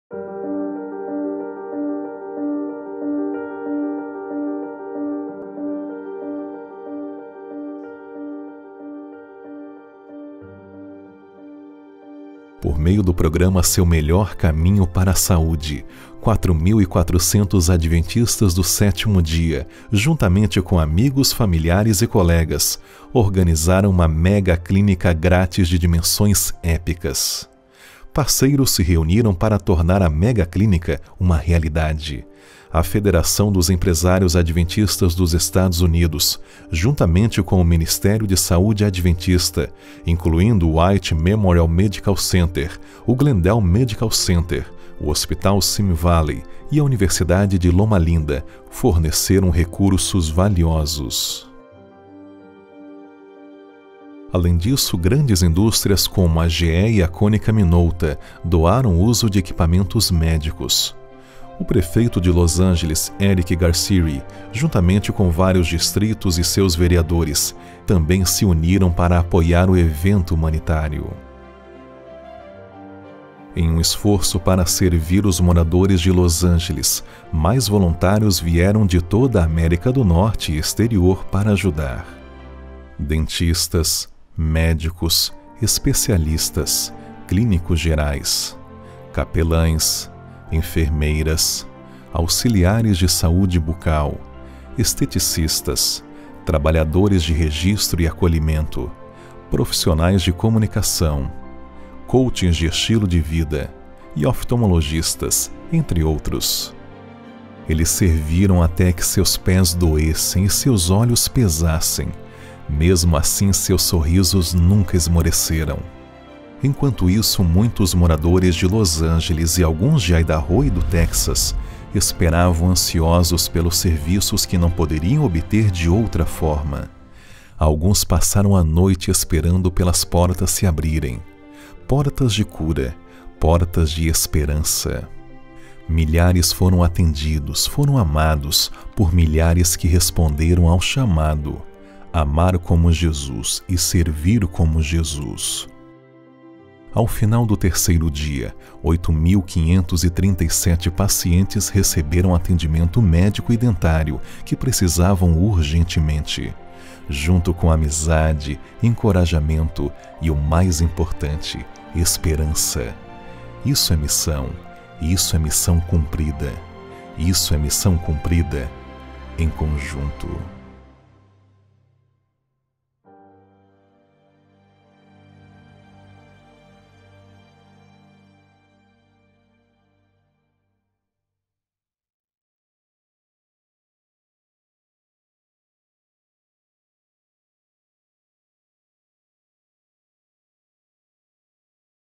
4º Sáb - Caminho para a saúde (Parte 2) 2º Trim 2017
Através de um serviço humanitário chamado, Melhor Caminho para a Saúde, 4.400 membros da Igreja Adventista do Sétimo Dia junto com amigos, familiares e colegas, e hospedam uma grande clínica gratuita em uma escala épica.